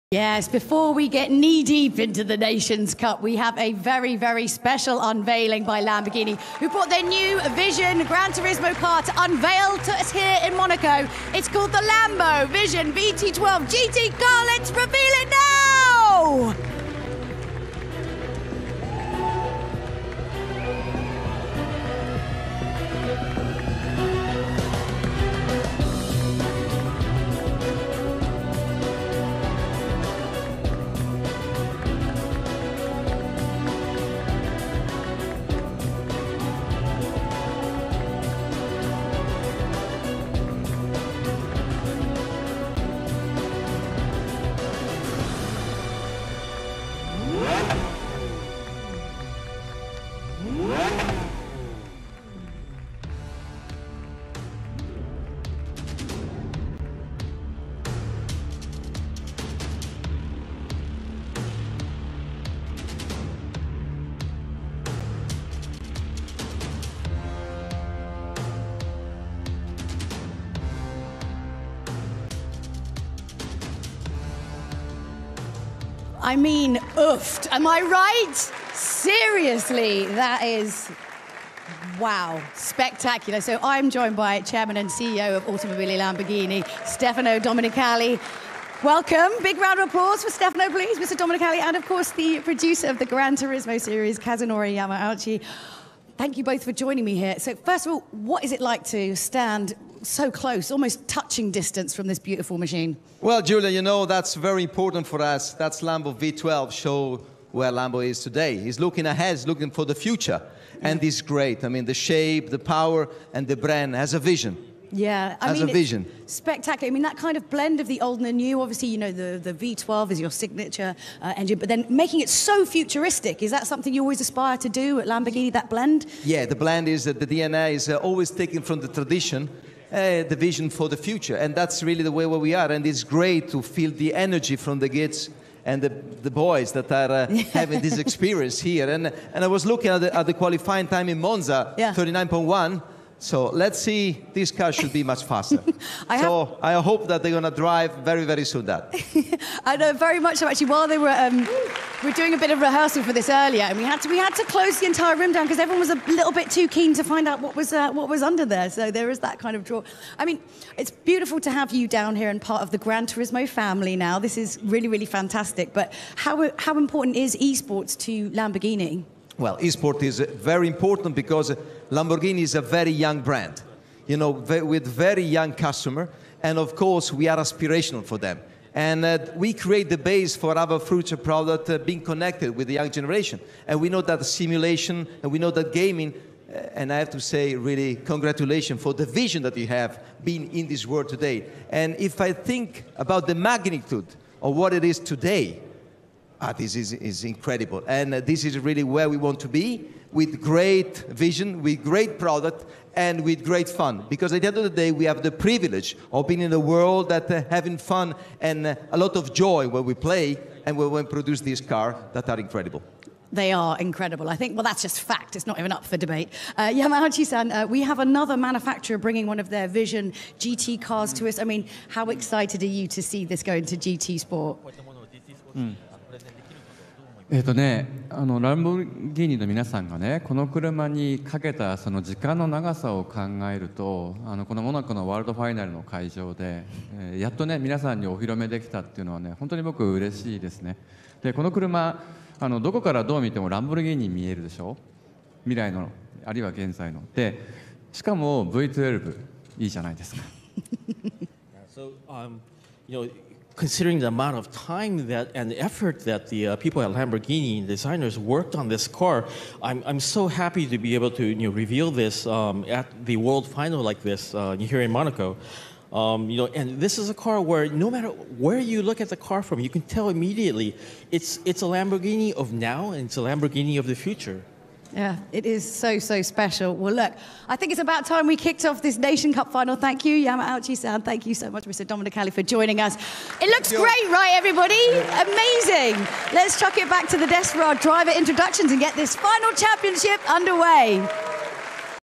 The Lamborghini Lambo V12 Vision Gran Turismo revealed at Gran Turismo Sport
At the 2019 world finals of the FIA-Certified Gran Turismo Championship in Monte Carlo, Lamborghini unveiled the V12 Vision Gran Turismo concept that was teased earlier this week. The prototype displayed there is, in fact, an actual single-seater, which will be available for gamers to drive in Gran Turismo Sport for PlayStation 4 from the spring of next year.
the V12 Vision Gran Turismo uses the same electrified powertrain that powers the Sian. The Italian company is not revealing more tech details about its latest creation but most of you can probably recall the Sian has a 6.5-liter V12 naturally aspirated engine accompanied by a 48-volt mild-hybrid system for a grand total of 819 horsepower (602 kilowatts). Lambo’s first-ever hybrid sprints from 0 to 62 miles per hour (0-100 kilometers per hour) in just 2.8 seconds and the top speed is electronically governed at 217 mph (350 kph).

As far as the new concept’s dynamic capabilities are concerned, we’ll have to wait until the car debuts for the Gran Turismo Sport to know more.